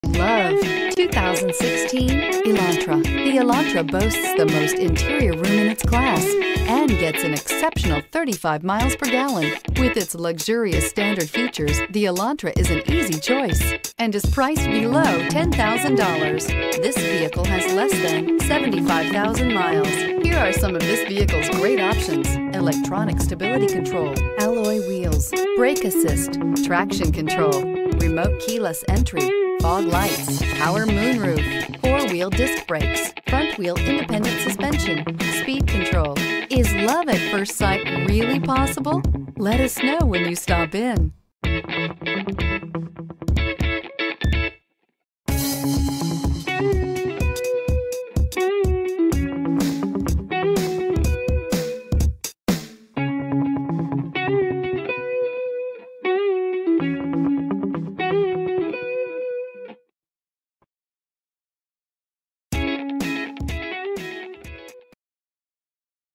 2016 Hyundai Elantra Melbourne FL H71588B
2016 Hyundai Elantra Value Edition

This 2016 Hyundai Elantra Value Edition in Scarlet Red Pearl features: FWD Clean CARFAX. 28/38 City/Highway MPG Awards: * 2016 KBB.com 10 Best Used Compact Cars Under $15,000 * 2016 KBB.com Best Buy Awards Finalist * 2016 KBB.com 5-Year Cost to Own Awards Visit our virtual showroom 24/7 @